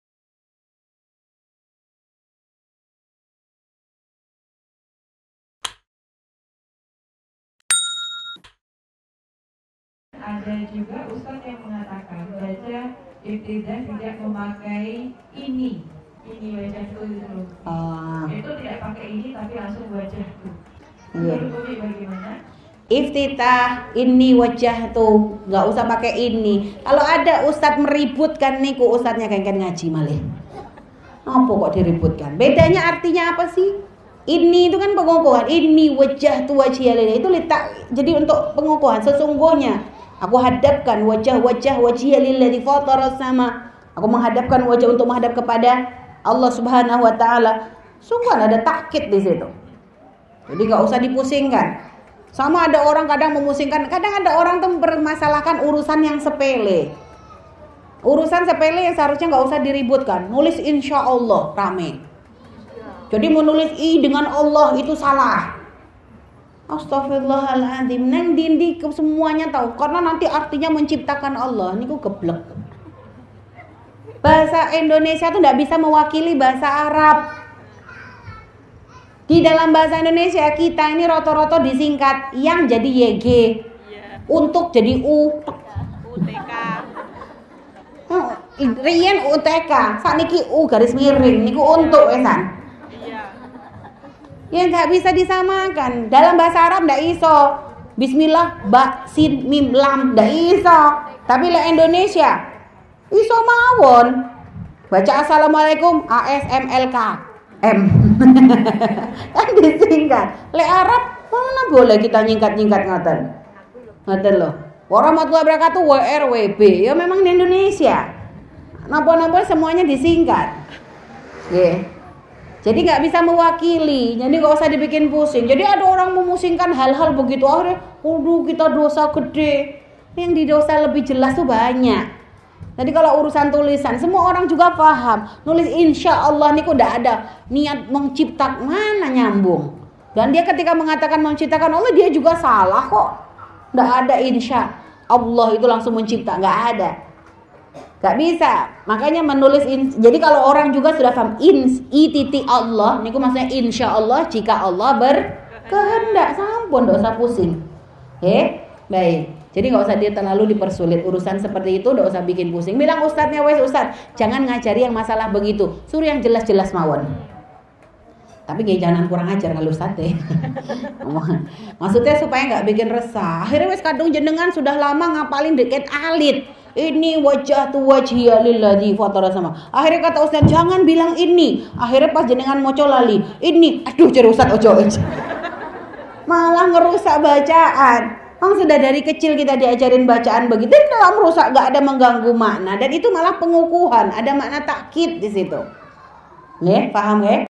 Bacaan Iftitah yang Benar - Ummi Fairuz Ar-Rahbini Menjawab
Ummi Fairuz Ar-Rahbini (istri Buya Yahya) official

Bacaan iftitah adalah doa yang dibaca setelah takbiratul ihram, sebelum membaca Surat Al Fatihah. Bagaimanakah bacaan iftitah yang benar itu? mari simak penjelasan Ummi Fairuz berikut ini :

INFORMASI INFAQ CENTRE AL-BAHJAH :

No. Rek : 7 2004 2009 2
KODE BANK : 451
a/n : Yayasan Al Bahjah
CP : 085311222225